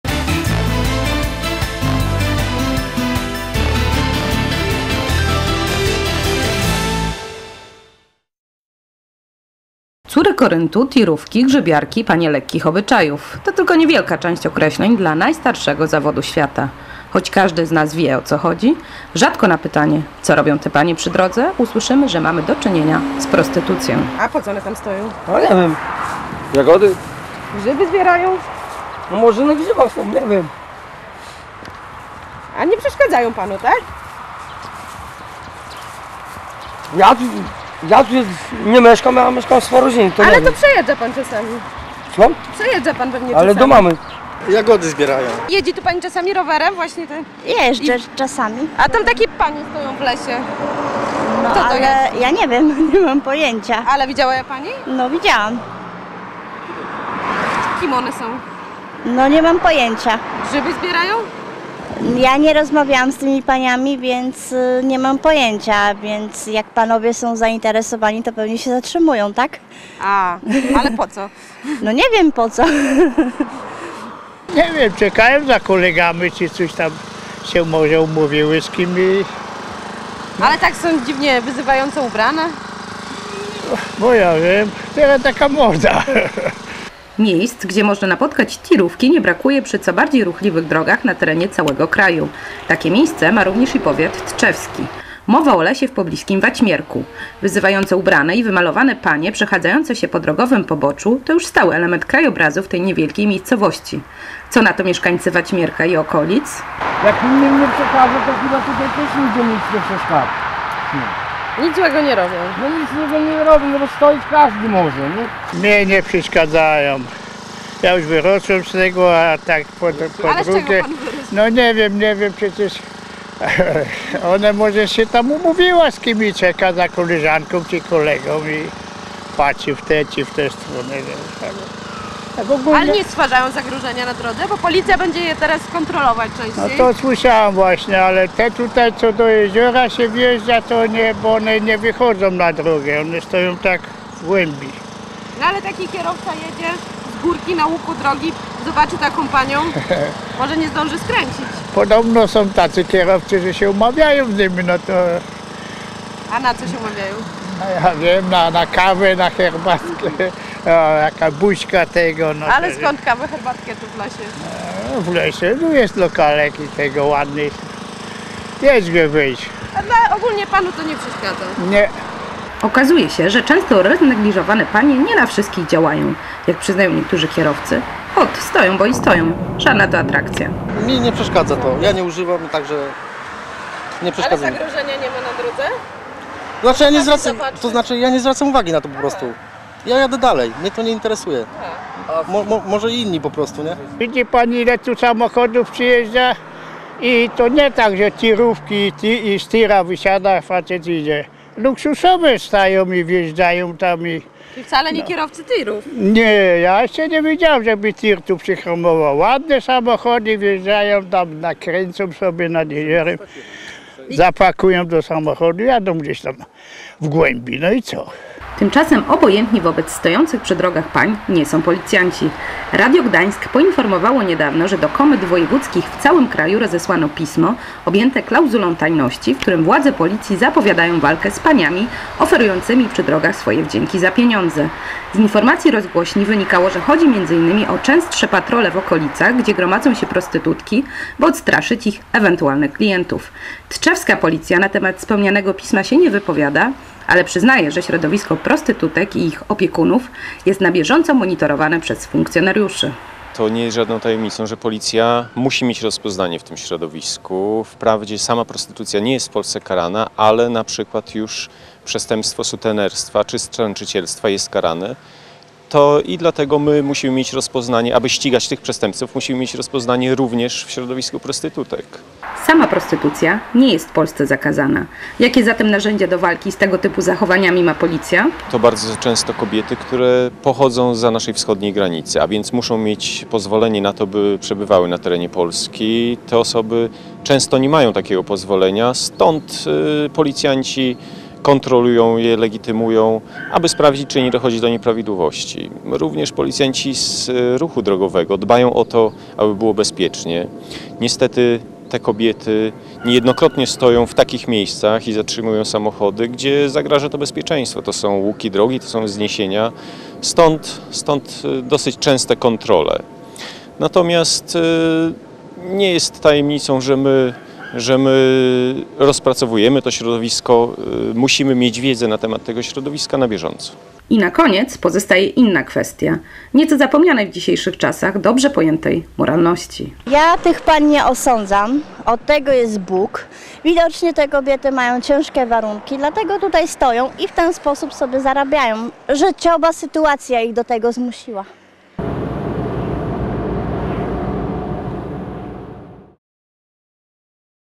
Policja idzie na wojnę z "tirówkami"
Córy Koryntu, tirówki, grzybiarki, panie lekkich obyczajów - to tylko niewielka część określeń dla najstarszego zawodu świata. Choć każdy z nas wie, o co chodzi - rzadko na pytanie - "co robią te panie przy drodze", usłyszymy, że mamy do czynienia z prostytucją. Miejsc, gdzie można napotkać "tirówki" nie brakuje przy co bardziej ruchliwych drogach na terenie całego kraju. Takie miejsce ma również i powiat tczewski. Mowa o lesie w pobliskim Waćmierku. Wyzywająco ubrane i wymalowane panie przechadzające się po drogowym poboczu, to już stały element krajobrazu w tej niewielkiej miejscowości. Co na mieszkańcy Waćmierka i okolic? Okazuje się, że często roznegliżowane panie nie na wszystkich działają. Jak przyznają niektórzy kierowcy - ot stoją, bo i stoją - żadna to atrakcja. Tymczasem obojętni wobec stojących przy drogach pań nie są policjanci. Radio Gdańsk poinformowało niedawno, że do komend wojewódzkich w całym kraju rozesłano pismo, objęte klauzulą tajności, w którym władze policji zapowiadają walkę z paniami oferującymi przy drogach swoje wdzięki za pieniądze. Z informacji rozgłośni wynikało, że chodzi m. in. o częstsze patrole w okolicach, gdzie gromadzą się prostytutki, by odstraszyć ich ewentualnych klientów. Tczewska policja na temat wspomnianego pisma się nie wypowiada, ale przyznaje, że środowisko prostytutek i ich "opiekunów" jest na bieżąco monitorowane przez funkcjonariuszy. Sama prostytucja nie jest w Polsce zakazana, jakie zatem narzędzia do walki z tego typu zachowaniami ma policja? I na koniec pozostaje inna kwestia - nieco zapomnianej w dzisiejszych czasach dobrze pojętej moralności...